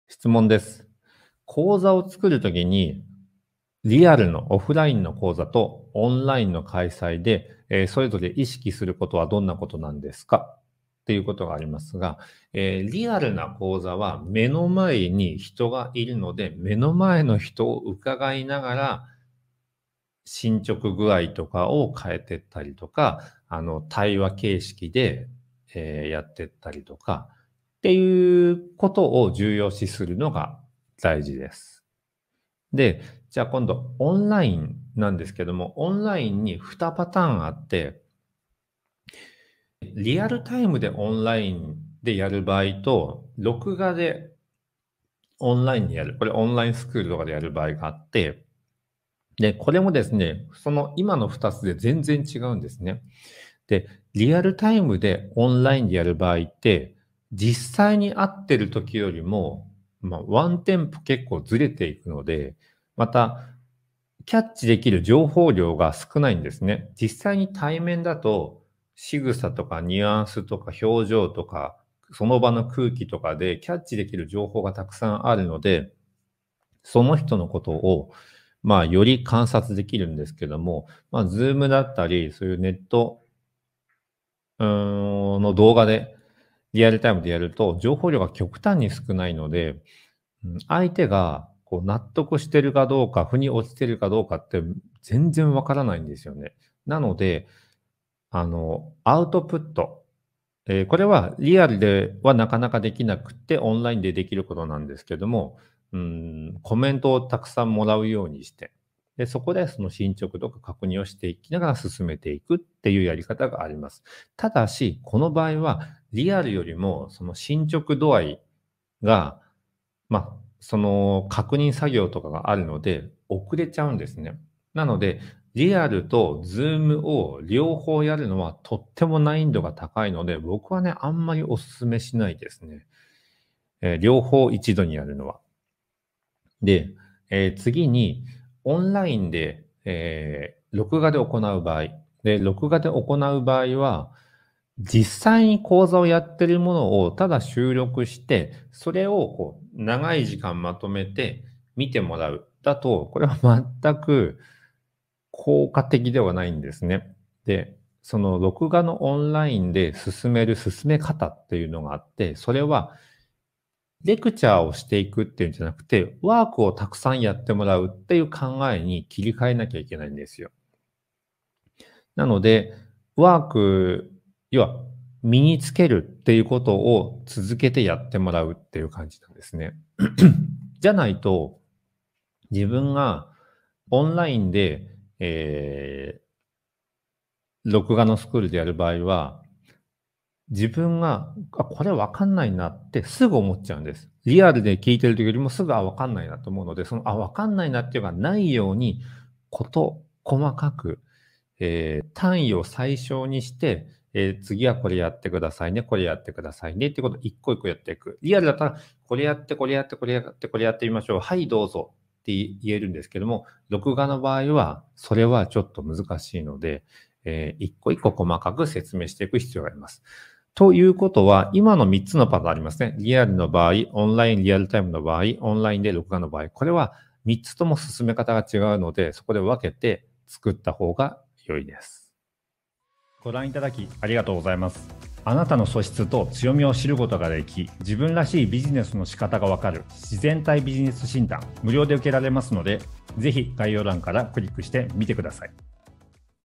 オンライン講座とオフライン講座の作り方の違いとは
▼「自分らしいビジネス」がわかる！
1分でわかるタイプ診断はこちら

この動画は、自分らしいビジネスを作るための
「ライフトラベラー サスティナブル ビジネスサロン」
のメンバーの方へコンサルティングしている様子をお届けしています。

▼マツダミヒロ著書籍『質問は人生を変える』プレゼント（メルマガ登録）

▼マツダミヒロWEBメディア（自分探しトラベル）